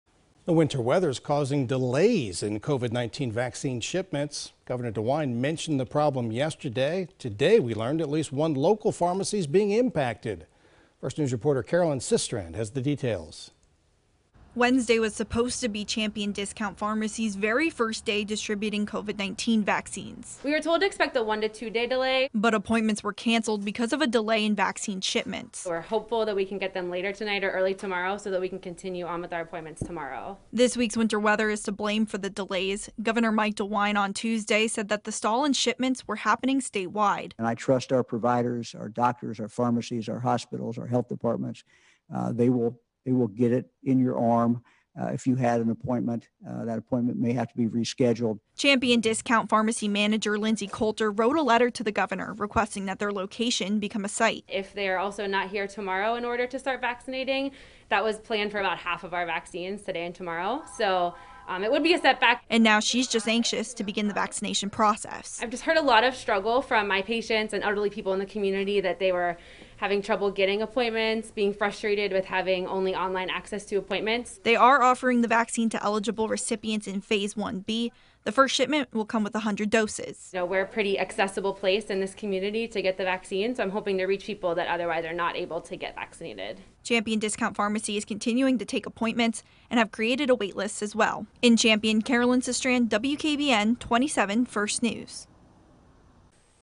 Champion pharmacy and others wait on delayed vaccine shipments
Wednesday was supposed to be the first day that Champion Discount Pharmacy distributed COVID-19 vaccines